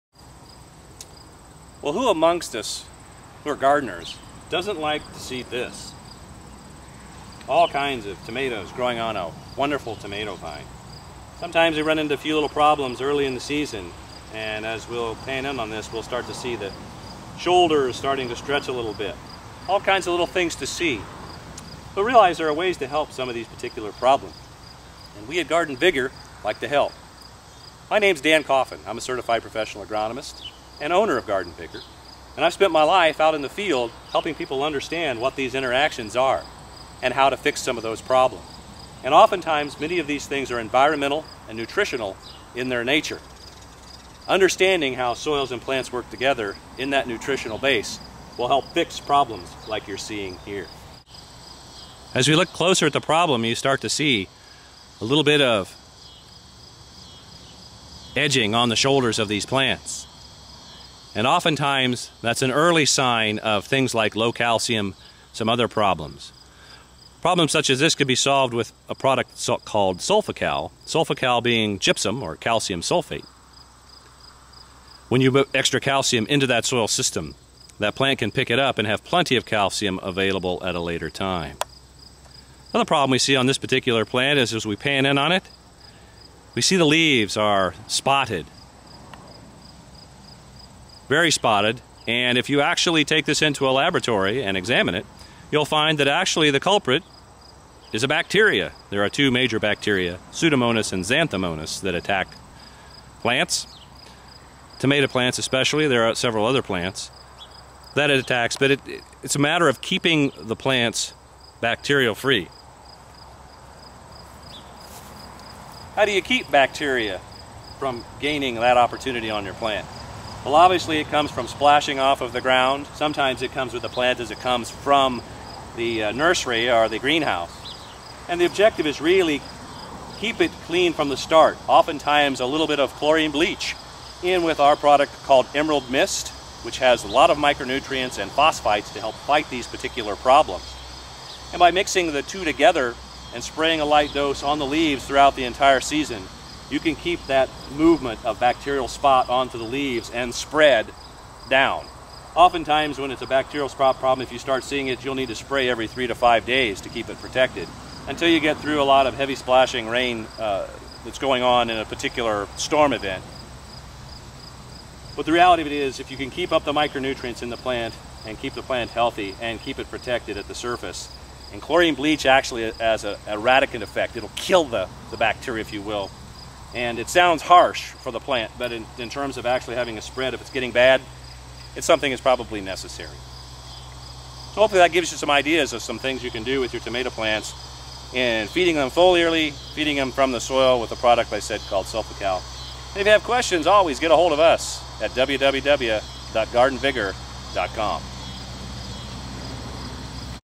Garden Vigor Helps
Dan talks about Garden Vigor philosophy and some tomato problems.